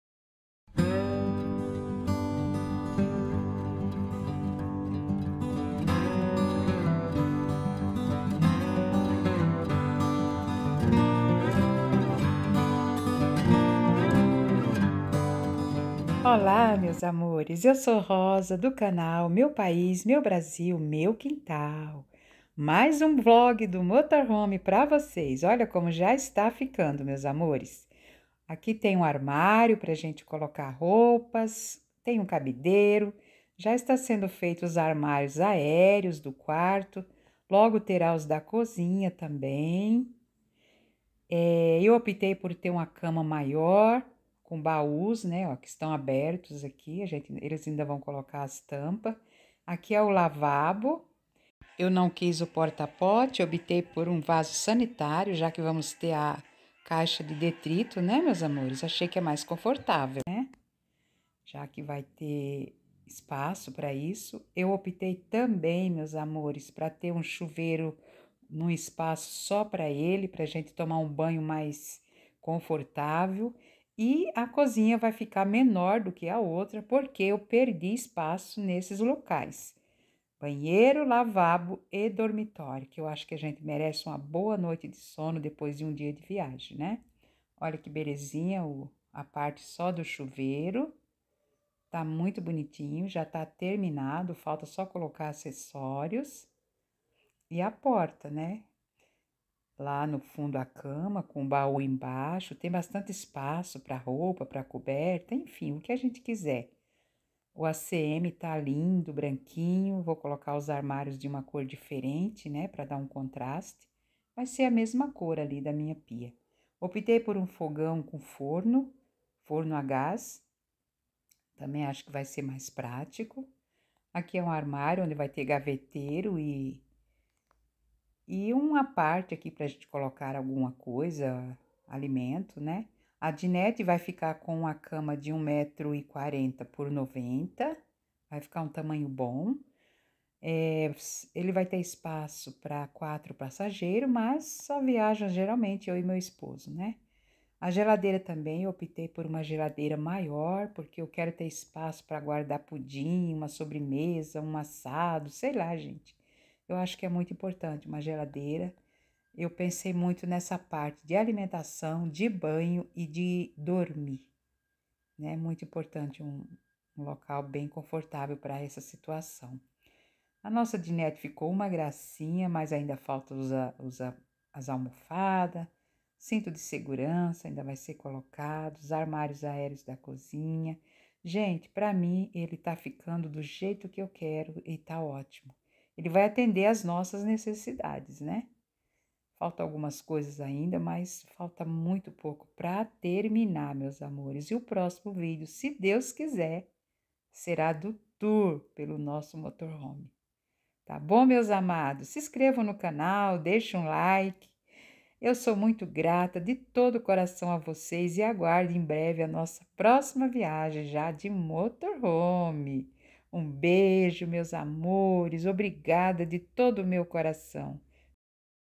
montando um motor home episódio 4
Olá meus amores. Mais um pouco da montagem do nosso motor home pra vcs . Logo estará pronto pra nossa próxima viagem.  Agradeço  a presença e o apoio de todos. Gratidão  sempre. ❤️